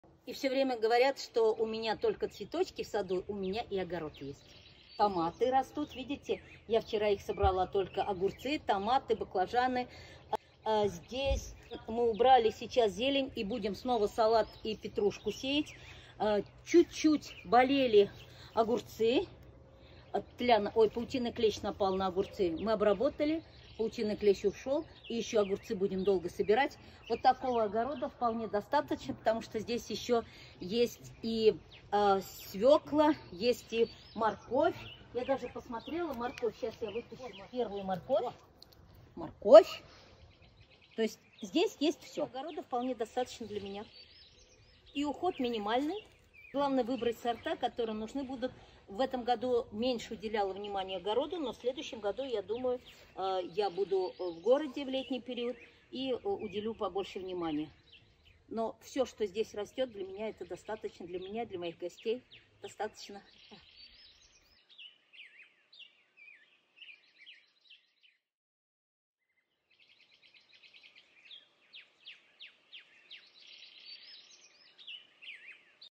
ВЫХОДНЫЕ НА ДАЧЕ 💛
Добрый день, дорогие и любимые садоводы!

В горячую летнюю пору у меня выходных конечно не бывает, но на даче я бываю регулярно.

По мимо цветов у меня конечно же есть и огород, на котором есть все необходимое. Здесь есть морковь и свекла, капуста и кабачки, томаты и огурцы. Есть конечно же и пряные травы, которые я очень люблю.

Этого вполне хватает мне и моим гостям.
Я не веду битву за урожай, а потом не веду битву с урожаем.😂
Все только в удовольствие!